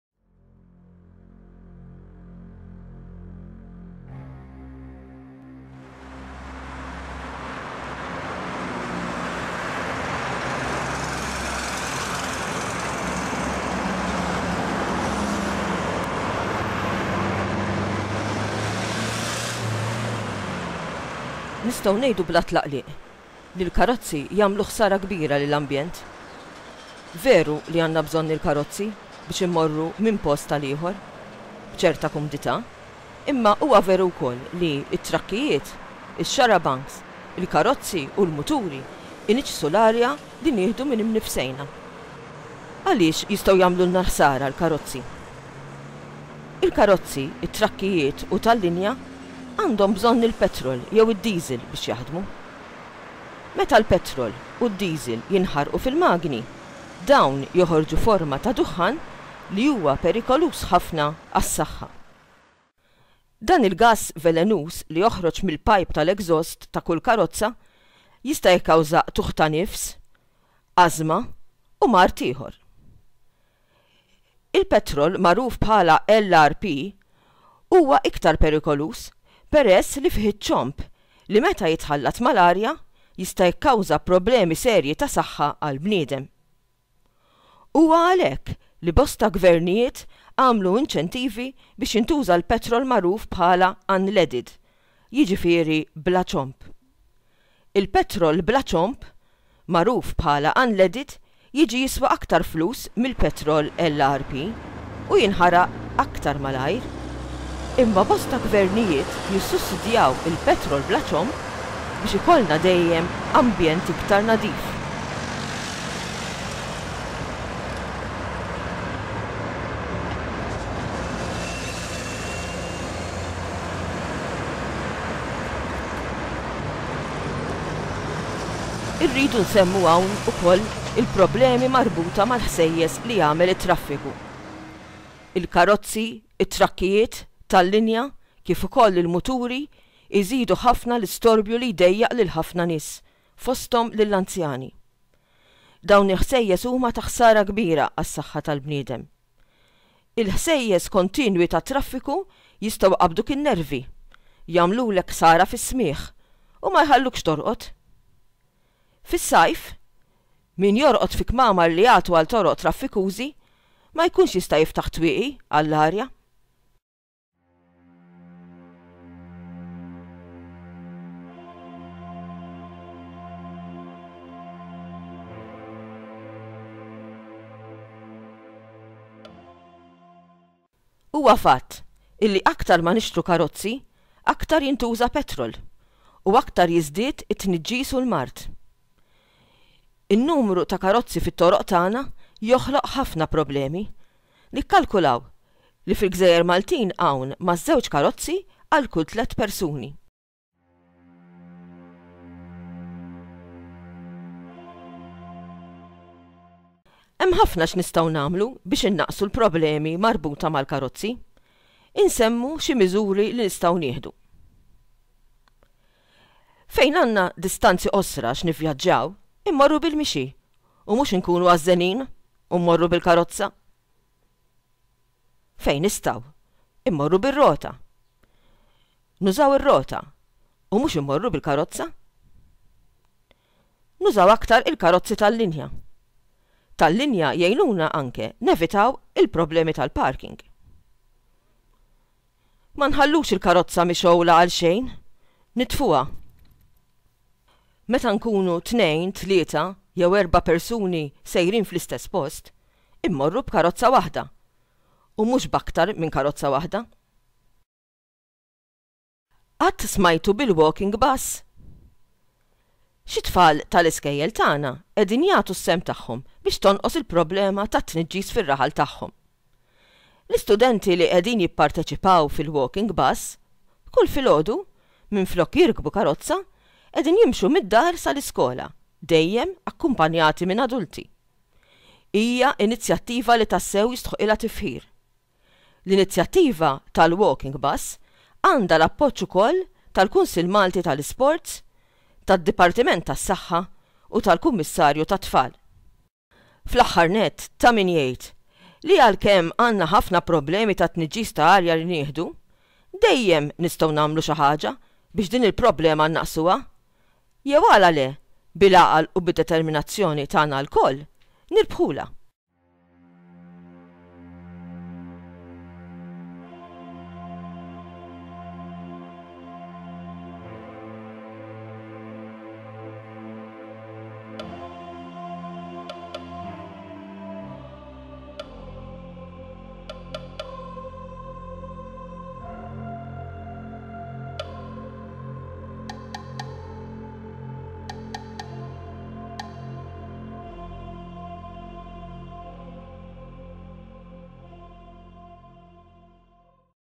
Tniggis mit-Traffiku
It-TNIĠĠIS MIT-TRAFFIKU - video edukattiv fis-sensiela Studji Soċjali; produzzjoni ta' Tony Pace. Ħajr lil Edward Gilson.